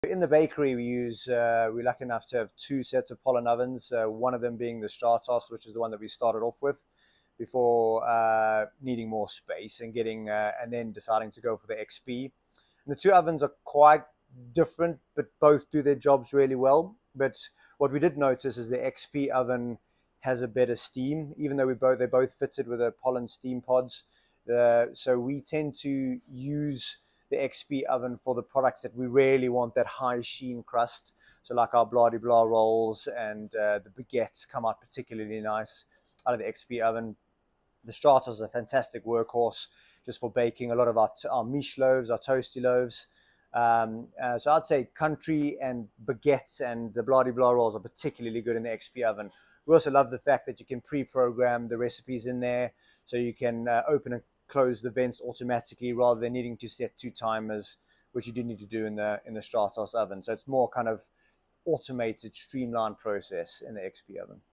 Tim speaks to us about his Polin XP and Stratos Deck Ovens
Tim Goodwin, owner of The Street Bakeshop, has both a Polin XP Deck Oven and a Polin Stratos Deck Oven so speaks to us about the differences he finds in their features. The XP Deck Oven is an upgraded version of the Stratos.